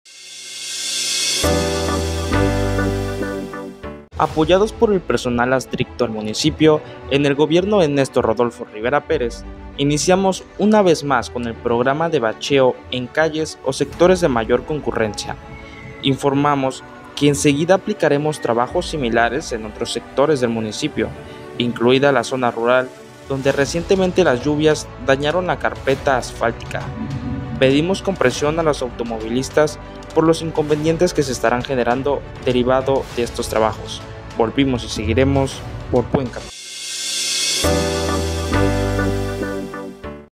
Apoyados por personal adscrito al municipio, en el gobierno de 𝗡𝗲́𝘀𝘁𝗼𝗿 𝗥𝗼𝗱𝗼𝗹𝗳𝗼 𝗥𝗶𝘃𝗲𝗿𝗮 𝗣𝗲́𝗿𝗲𝘇
Apoyados por personal adscrito al municipio, en el gobierno de 𝗡𝗲́𝘀𝘁𝗼𝗿 𝗥𝗼𝗱𝗼𝗹𝗳𝗼 𝗥𝗶𝘃𝗲𝗿𝗮 𝗣𝗲́𝗿𝗲𝘇 iniciamos una vez más con el programa de bacheo en calles o sectores de mayor concurrencia.
Informamos que en seguida aplicaremos trabajos similares en otros sectores del municipio, incluida la zona rural donde recientemente las lluvias dañaron la carpeta asfáltica.
Pedimos comprensión a los automovilistas por los inconvenientes que se estarán generando derivado de estos trabajos.